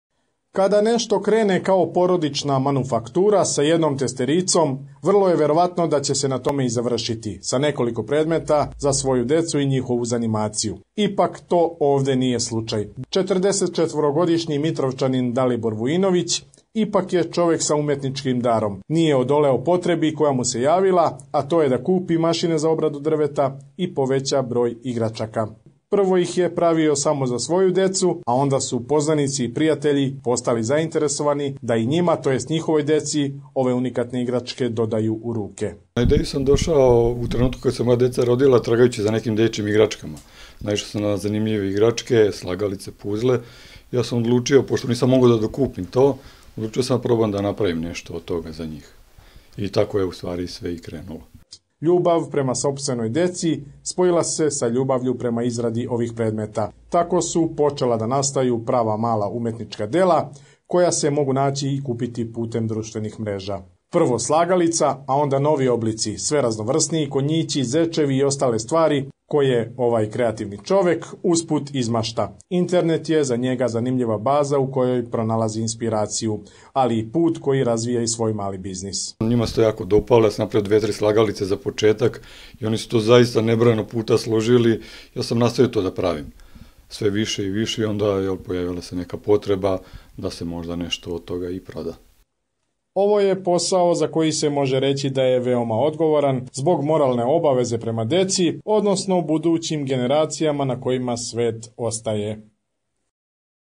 Drvene igracke SM 26.04.2017.
U moru novih tehnologija i savremenih igračaka još uvek ima onih koji se drže tradicije. Dalibor Vujinović iz Sremske Mitrovice već nekoliko godina unazad proizvodi ekološke igračke od drveta. Sve je počelo od želje da svojoj deci priušti kvalitetne i kreativne drvene igračke, tako da se u međuvremenu razvio i skroman biznis.